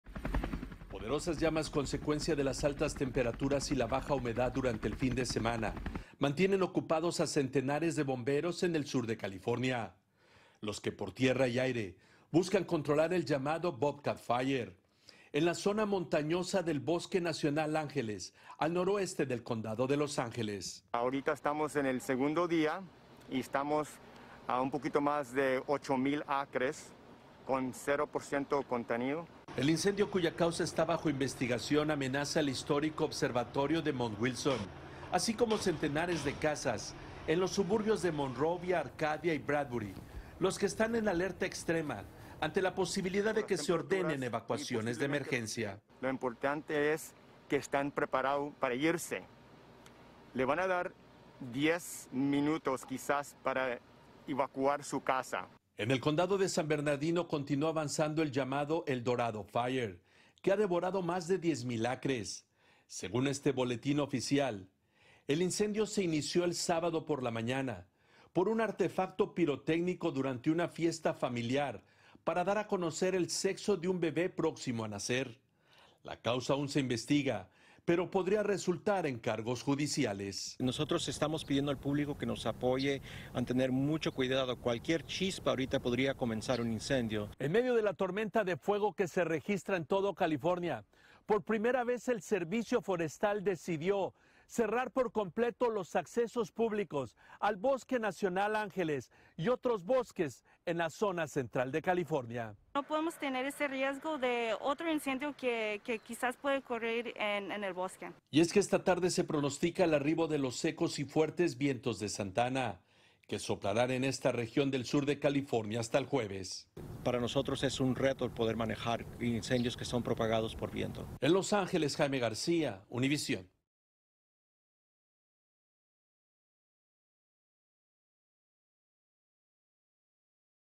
Por aire y tierra, cientos de bomberos tratan de controlar los incendios en California
Uno de los siniestros amenaza el observatorio de Monte Wilson, así como centenares de viviendas en suburbios de Monrovia, Arcadia y Bradbury. En el condado de San Bernardino otro incendio ha devorado más de 10,000 acres y, según un boletín oficial, fue causado por un artefacto pirotécnico durante una fiesta familiar para dar a conocer el género de un bebé. La llegada a la zona de los fuertes y secos vientos de Santa Ana preocupa a las autoridades.